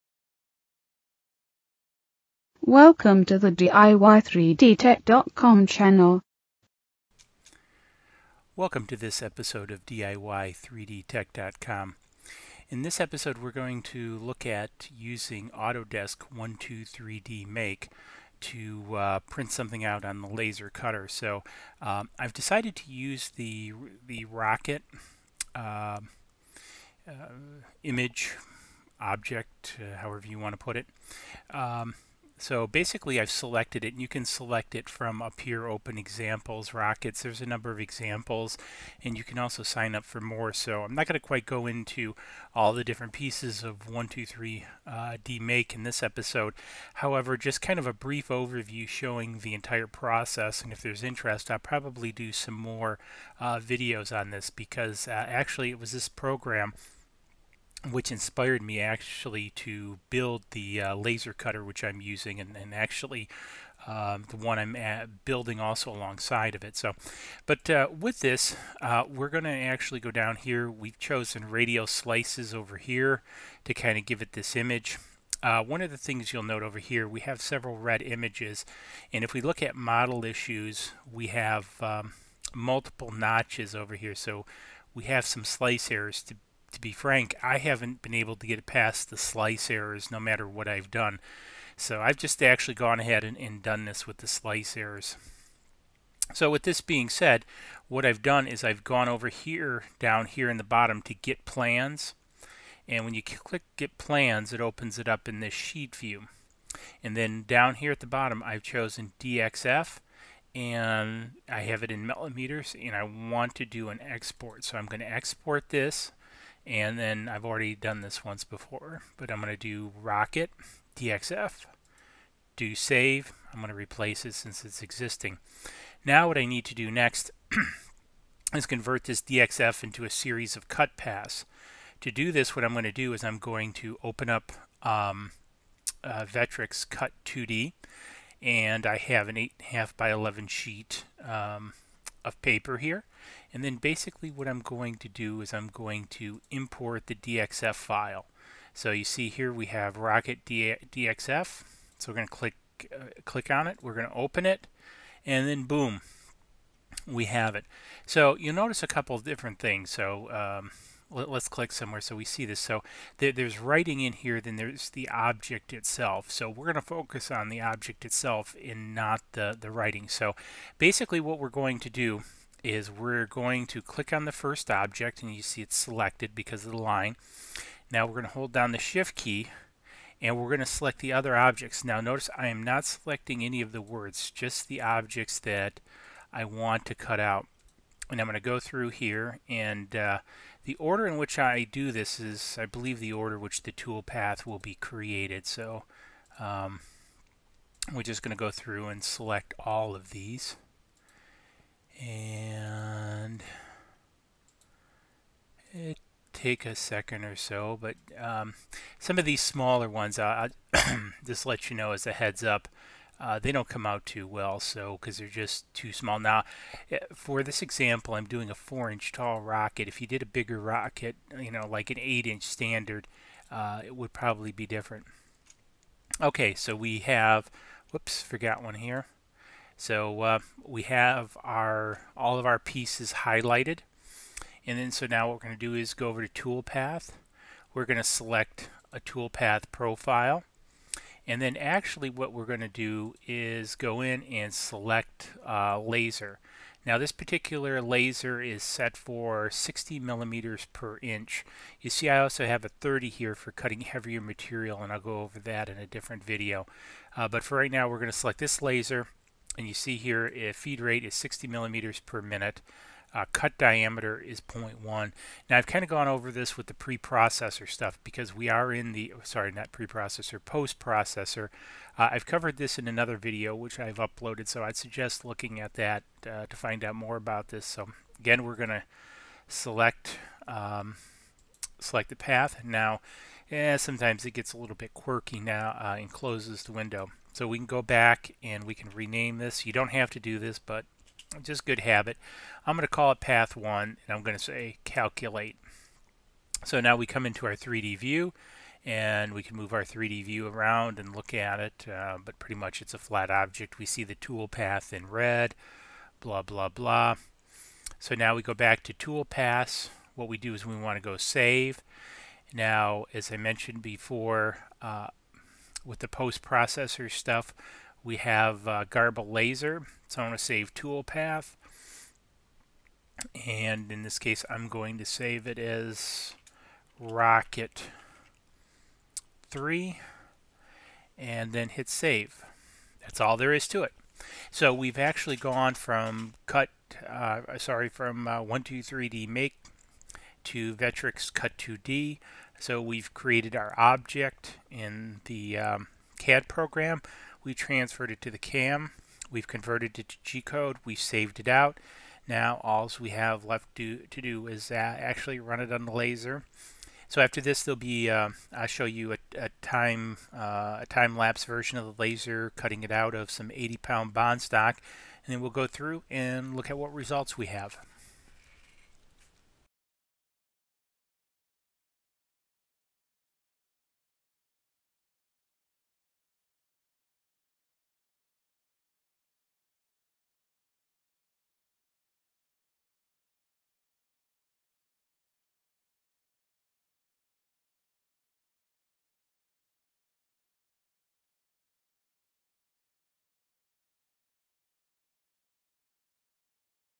Autodesk 123D Make Laser Cutting Rocket Model
In this episode we show you how to go from Autodesk 123D Make to Vectric Cut2D to create Gcode to a drive GRBL CNC router with a 2 Watt Laswer Diode Cutter.

Also remember safety first when using any tools and especially a laser!